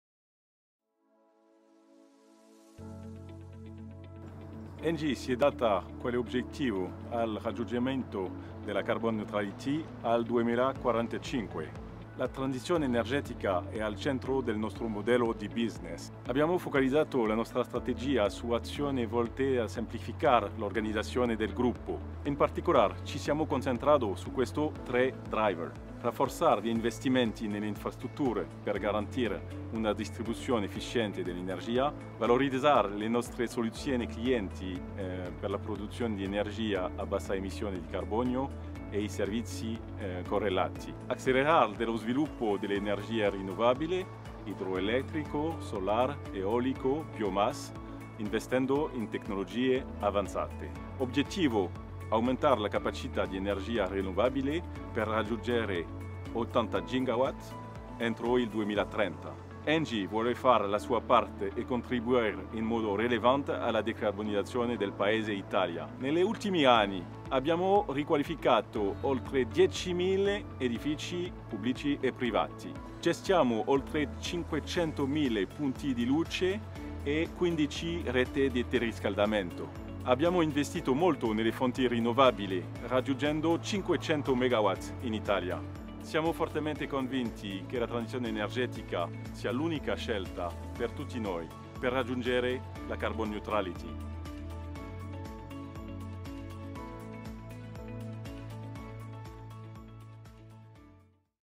Raggiungere la carbon neutrality entro il 2045 | Damien Térouanne
Dall'1 al 4 luglio 2021 si è tenuto a Marsala l'ENGIE Kitesurf Challenge, tappa del campionato nazionale di Kitesurf ma anche occasione per mostrare le meraviglie di una terra baciata dal sole ma anche dal vento.
A margine dell'evento, Damien Térouanne, CEO ENGIE Italia, ha raccontato l'obiettivo di ENGIE di raggiungere la carbon neutrality entro il 2045. Come?